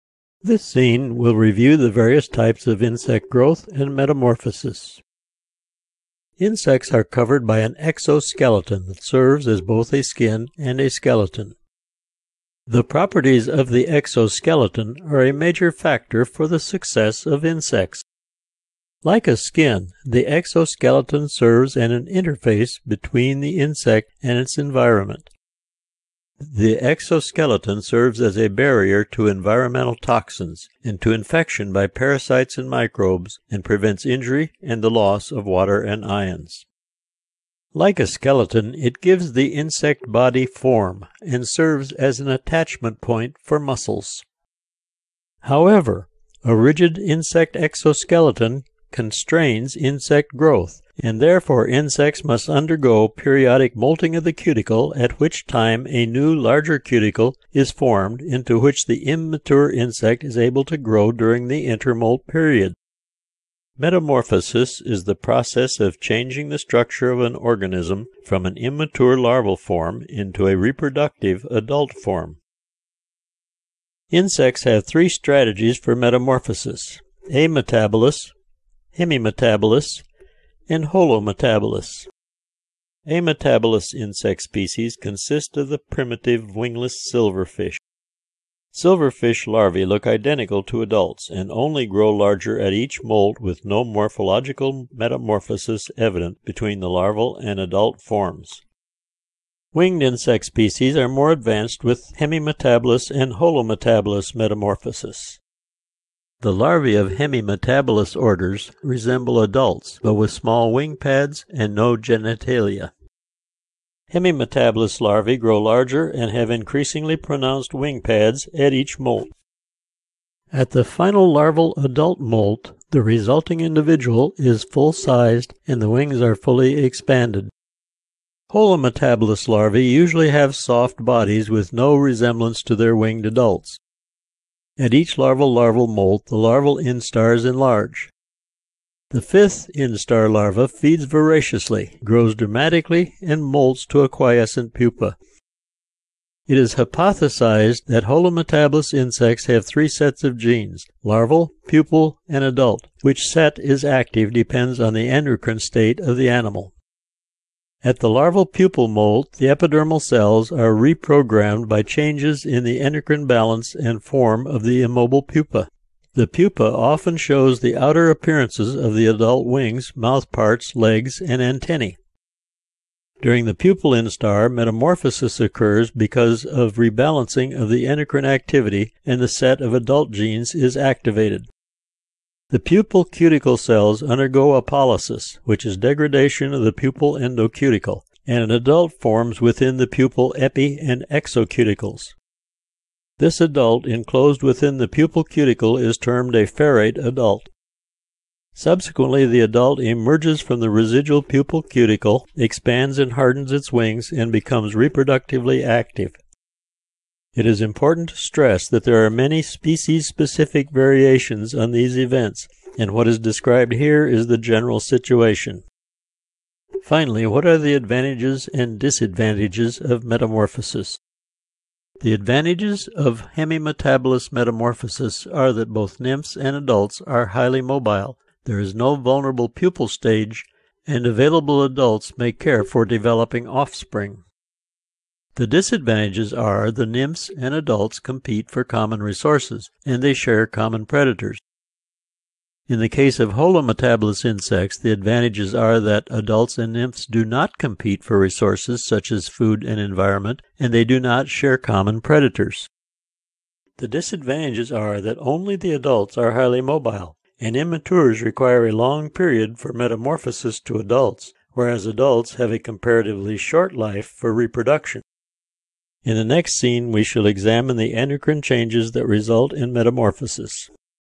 04-Growth-Metamorphosis
The role of the exoskeleton on insect growth and metamorphosis.